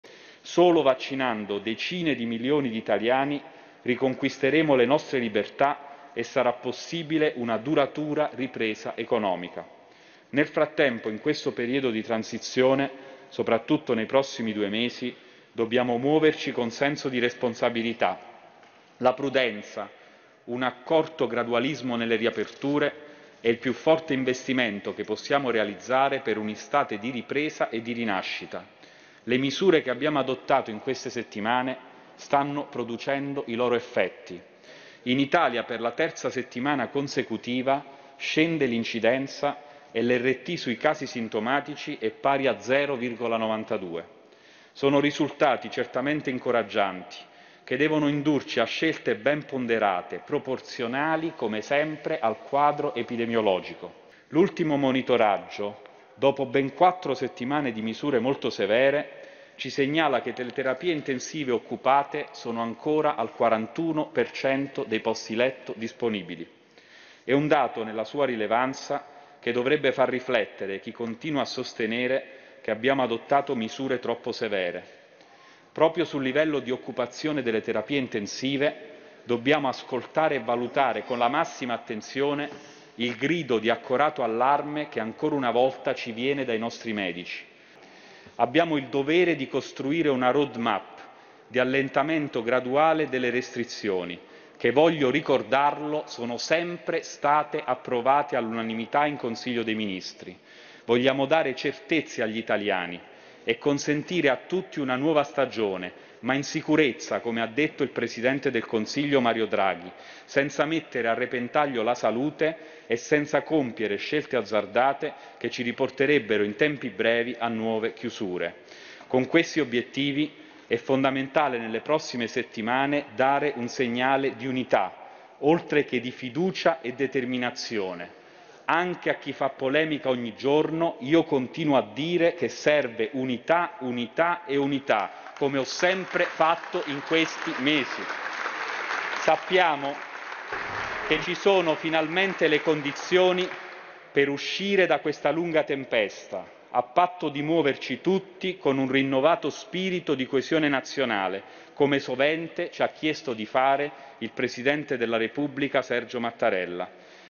Coronavirus, Speranza: "Misure troppo severe? 41% terapie intensive occupate. Serve unità"
Prudenza e ottimismo. Il ministro della Salute, Roberto Speranza, apre alla possibilità che si allentino le misure restrittive, ma difende la linea di severità applicata finora: "L'ultimo monitoraggio dopo 4 settimane di misure segnala che le terapie intensive sono ancora al 41% di occupazione: è un dato che dovrebbe far riflettere". Nell'informativa urgente alla Camera il ministro aggiunge: "Dobbiamo ascoltare il grido d'allarme dei medici. Dobbiamo essere tempestivi nelle chiusure quando serve. Ma ci sono le condizioni per uscire da questa lunga tempesta". .di Camilla Romana Bruno .